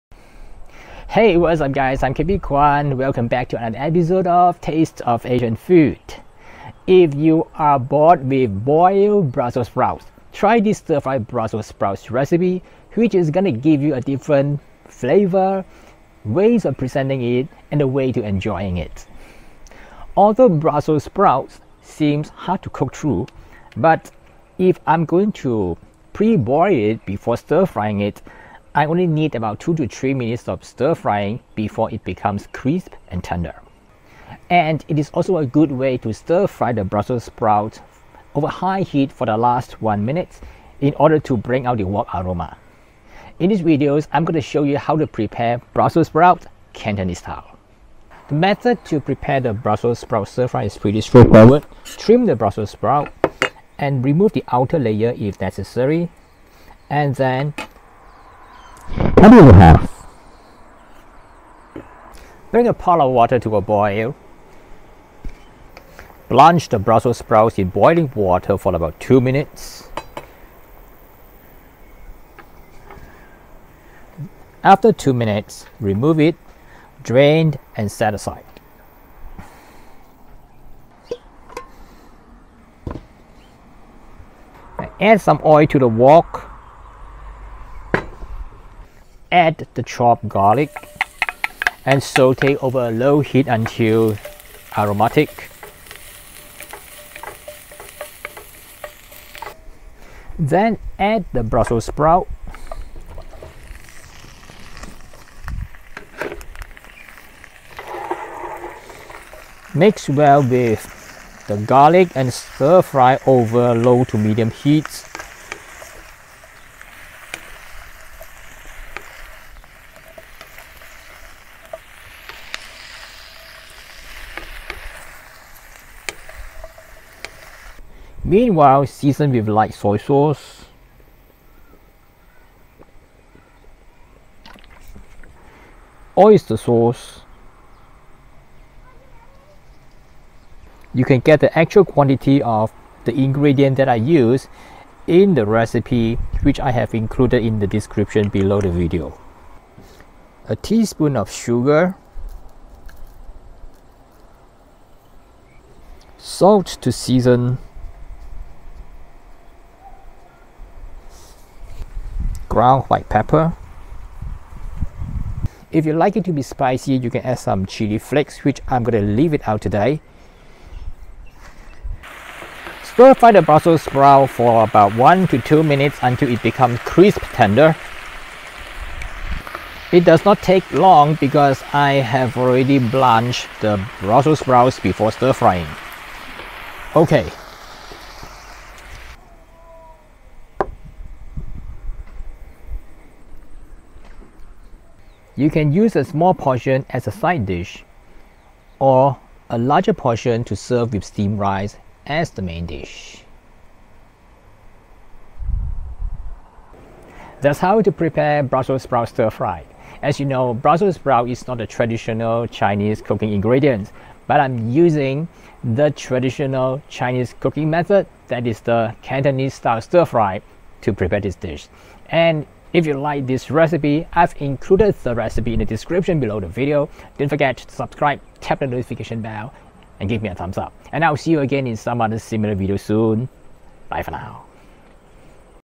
Brussels sprouts stir-fry recipe with garlic (Chinese style)
If you're tired of boiled Brussels sprouts, try this stir-fry recipe for a different and flavorful way to enjoy them.
Although Brussels sprouts may take time to cook due to their multiple layers, a quick pre-boil for 2 minutes followed by a high-heat stir-fry will produce a crunchy texture with a delightful wok aroma. It tastes far better than boiled Brussels sprouts. It's a perfect side dish for roast pork or other meat dishes.
In this video, I want to share a stir-fried Brussels sprouts recipe with an Asian flavor. As a bonus, I will also include a spicy Sichuan version. So, let's dive right into the details of both recipes!

Ingredients:
500g Brussels sprouts, trimmed and halved
2 tbsp vegetable oil
4 cloves garlic, coarsely chopped
1 tbsp light soy sauce
1 tbsp  oyster sauce
1/2 tsp sugar
1 tsp sesame oil
1/2 tsp salt
1/4 tsp ground white pepper
Red pepper flakes, to taste (optional)

Method:
​Begin by trimming the fresh Brussels sprouts and halving them.
Next, boil a pot of water and blanch the Brussels sprouts for two minutes. Drain in a colander and set aside.
Coarsely chop or mince four cloves of garlic.
Begin by heating vegetable oil in a wok. Once hot, add the minced garlic and saute until fragrant.
Next, add the drained Brussels sprouts and stir-fry over medium heat. Season the vegetable with light soy sauce, oyster sauce, sugar, salt, and pepper.
Continue stir-frying the Brussels sprouts over medium-high heat for about a minute or until they are crisp-tender and fragrant with the wok aroma. Then, drizzle sesame oil over the Brussels sprouts and mix well.

Note:
Ingredients for the Sichuan Brussels sprouts stir-fry:

600g Brussels sprouts, trimmed and halved
2 tablespoons Sichuan chili oil (Lao Gab Ma)
4 cloves garlic, minced
2 teaspoons ginger, grated
2-3 dried red chilies
2 tablespoons soy sauce
2 tsp rice vinegar
1 teaspoon sugar
Salt and pepper, to taste
Chopped scallions for garnish